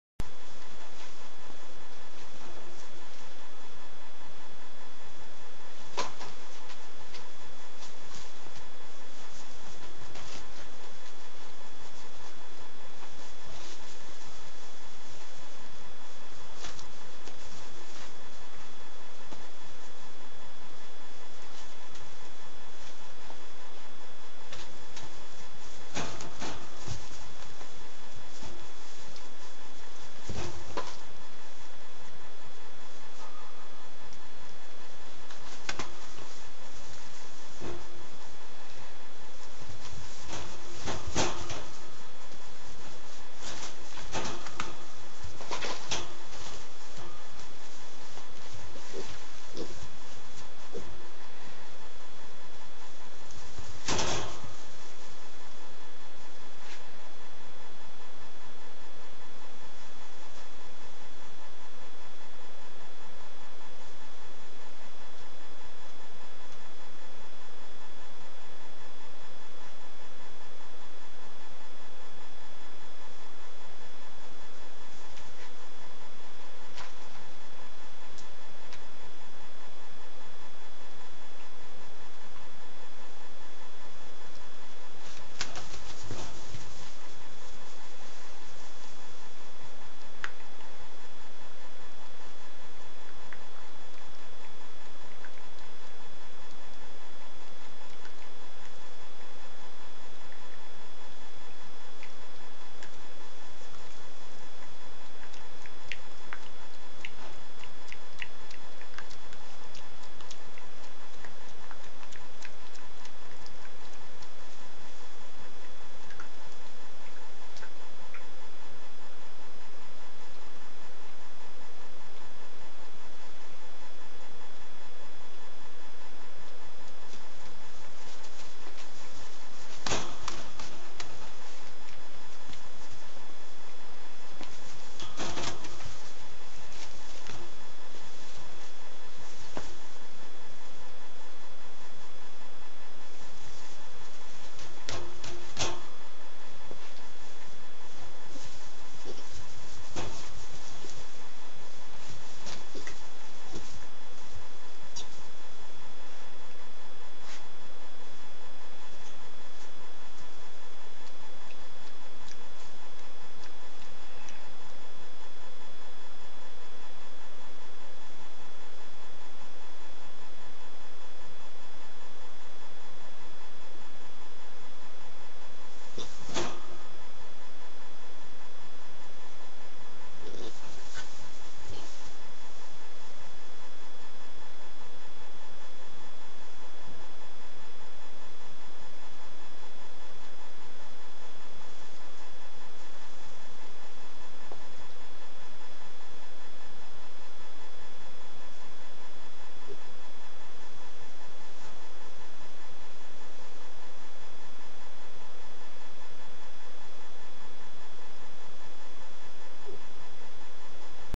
piglets playing with ball
piglets playing with ball, they like it more than my dog does.